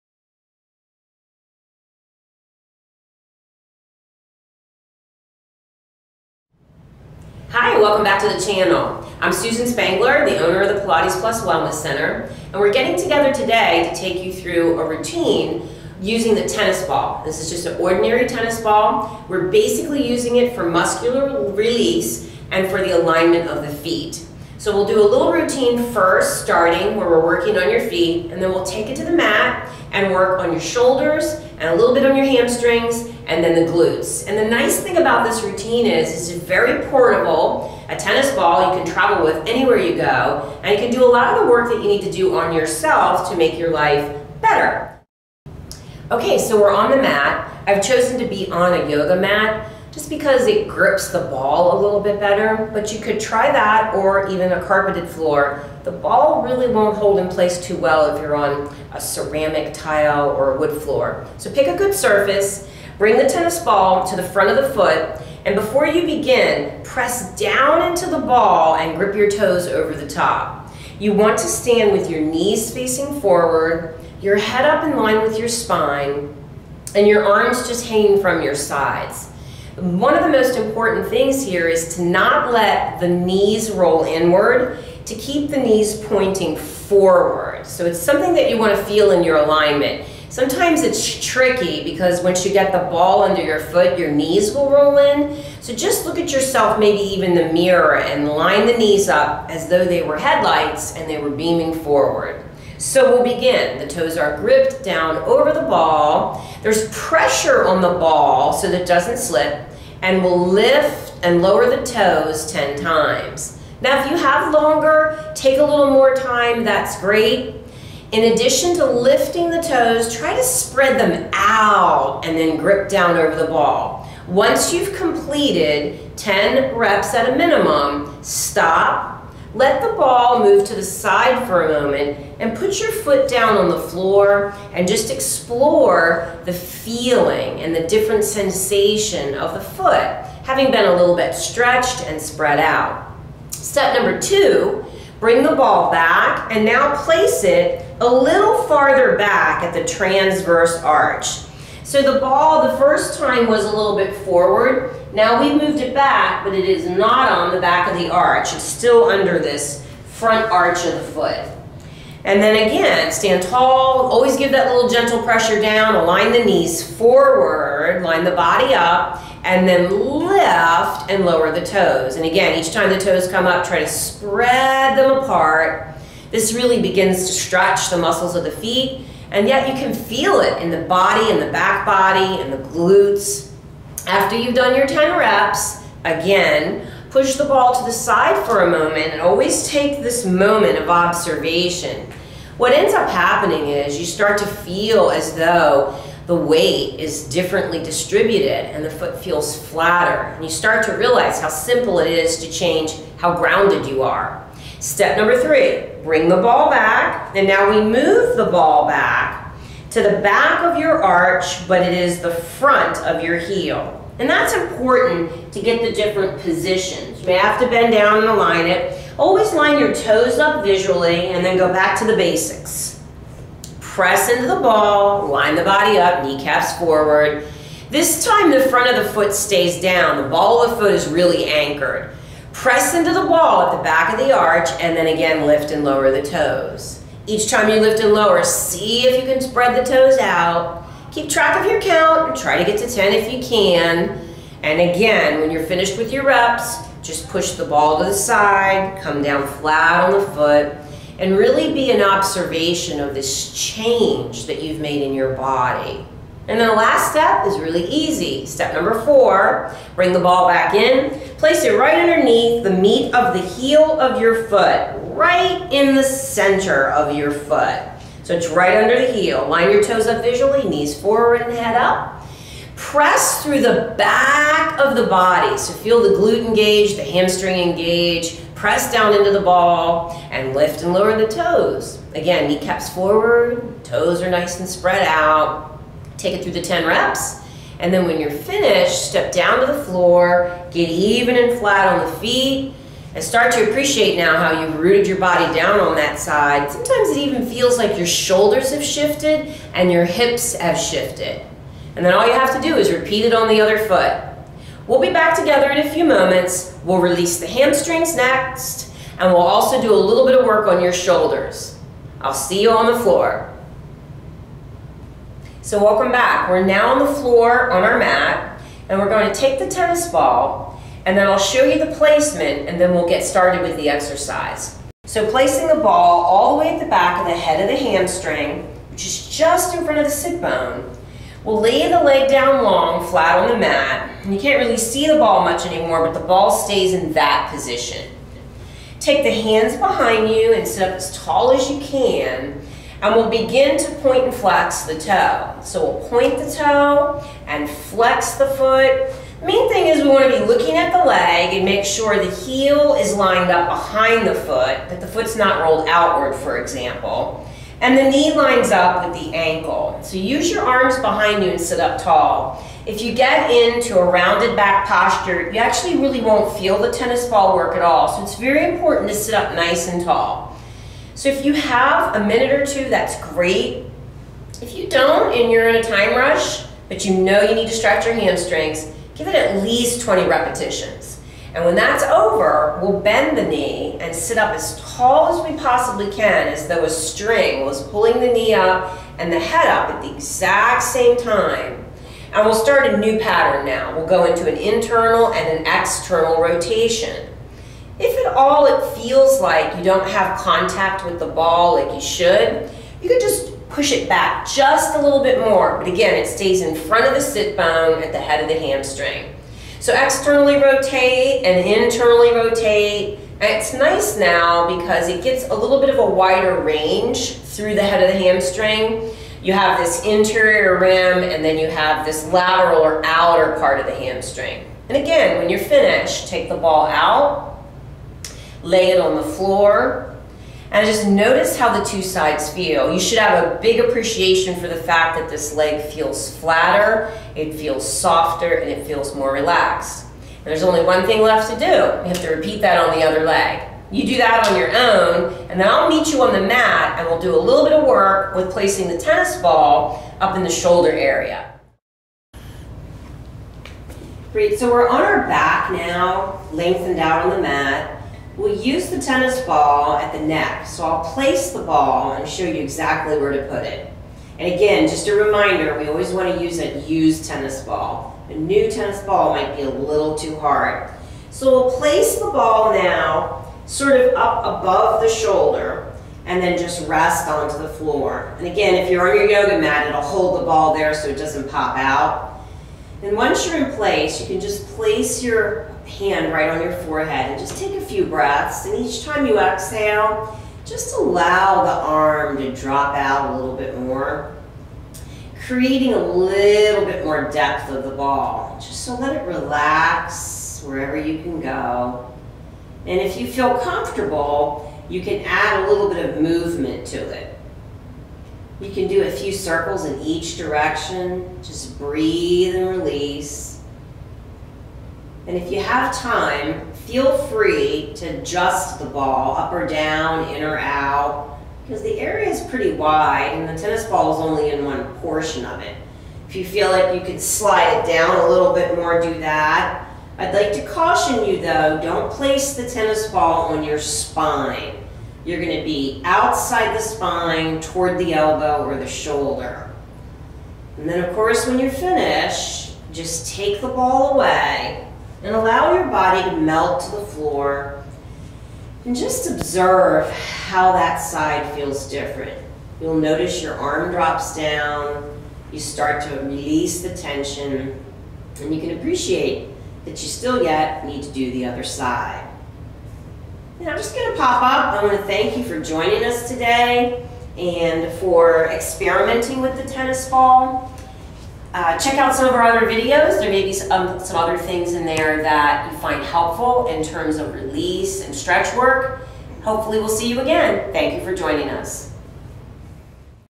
Tennis Ball Workout
This video will help you relieve tension, release muscles, and stretch feet.  Releasing the muscles of the feet helps with leg alignment and balance.  REMINDER:  Always use a USED tennis ball.